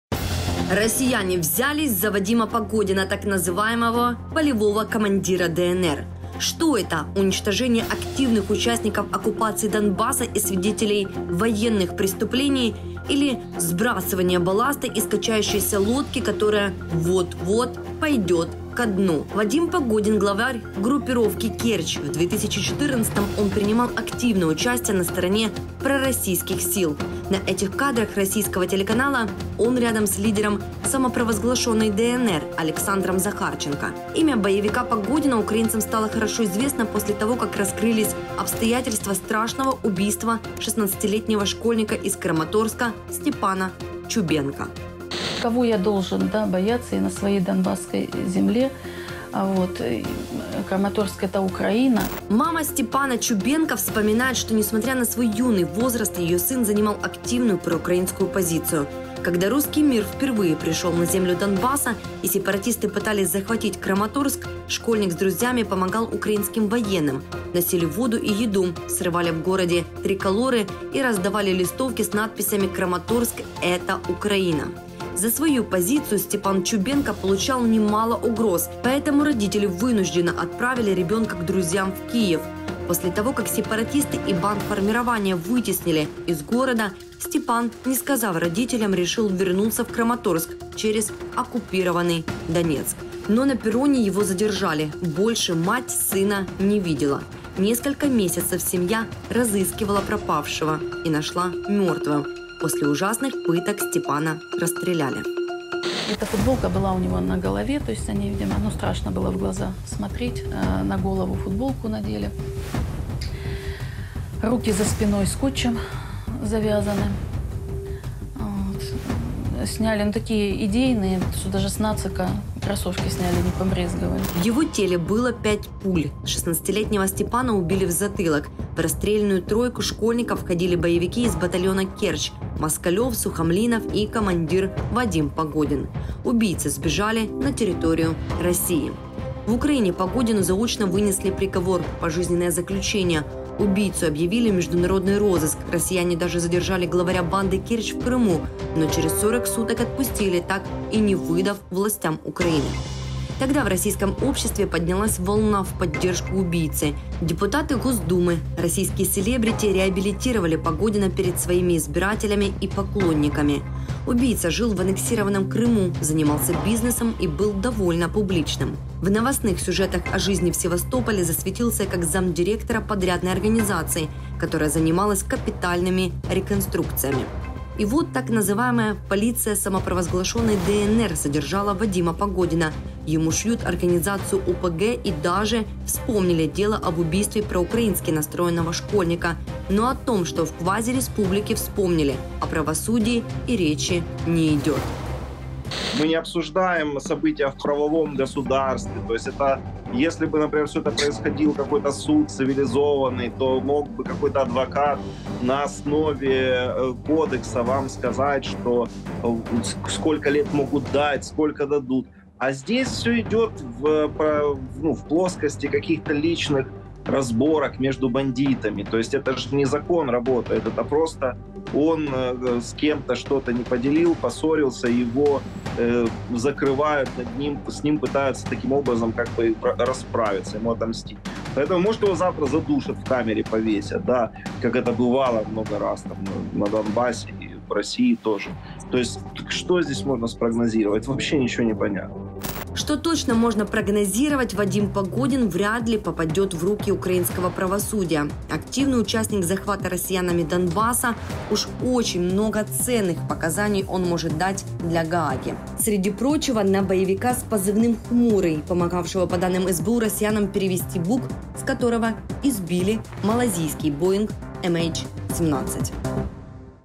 ⚡Одного из главных сепаратистов и убийц детей ДНР призвали к ответу
В 2014 году сепаратисты из т.н. ДНР зверски убили 16-летнего Степу Чубенко за его патриотическую позицию. Его убийца до недавних пор вольготно чувствовал себя на оккупированной территории и в рф. Но неожиданно, спустя много лет, его призвали к ответу. Расскажем подробности этой запутанной истории!